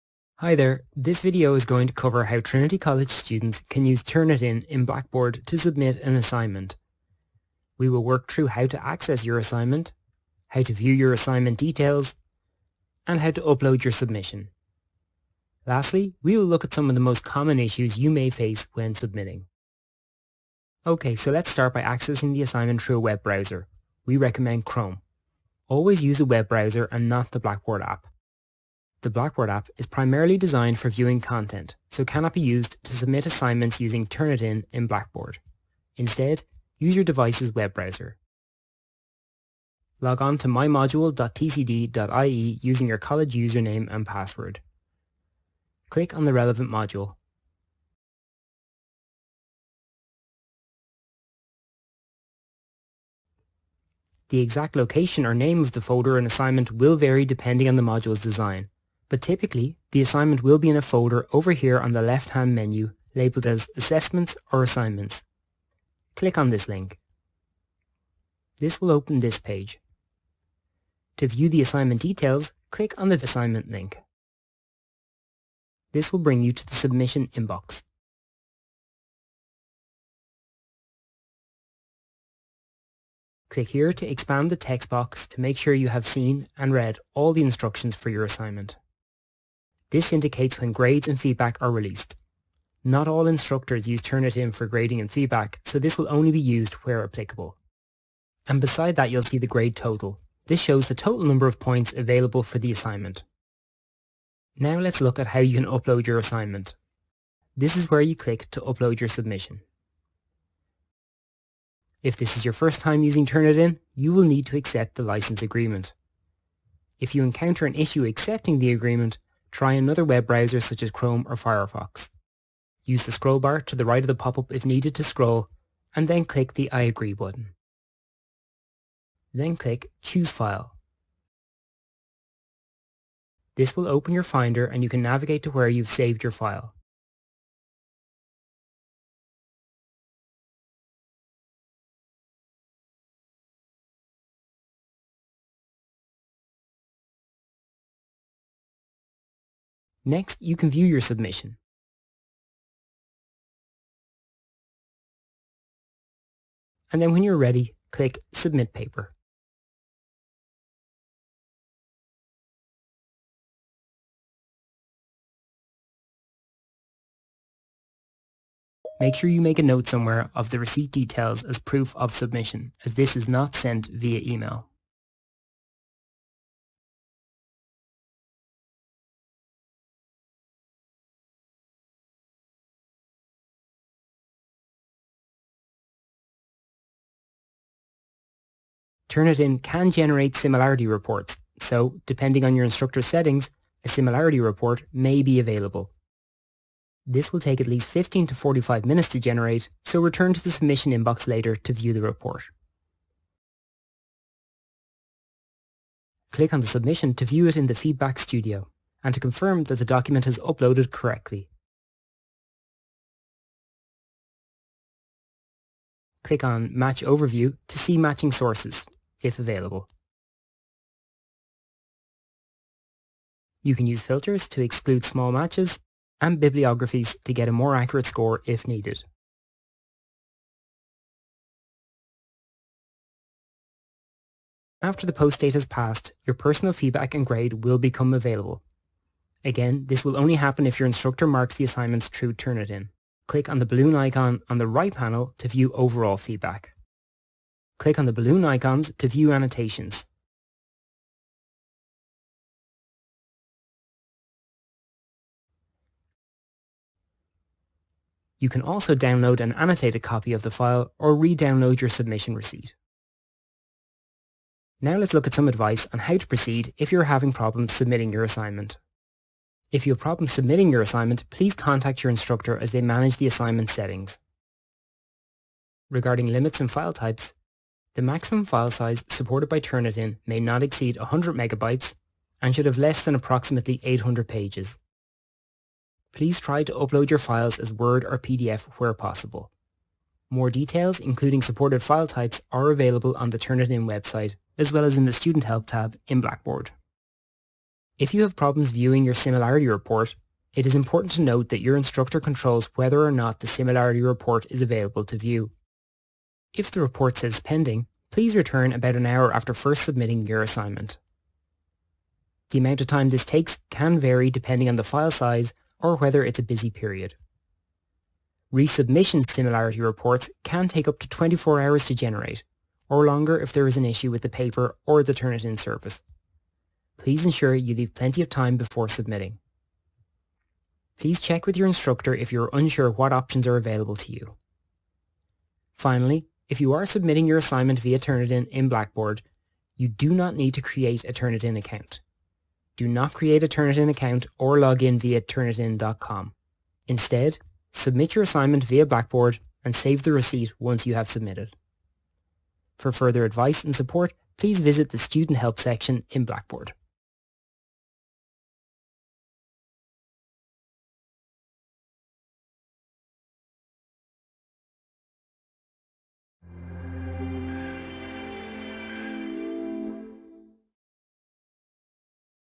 Turnitin in Blackboard - Student Guide
This video gives an overview of how Trinity College Dublin students can access and submit assignments to Turnitin in Blackboard (mymodule.tcd.ie). Click on the Student Help tab when you log in to Blackboard for more guides and information.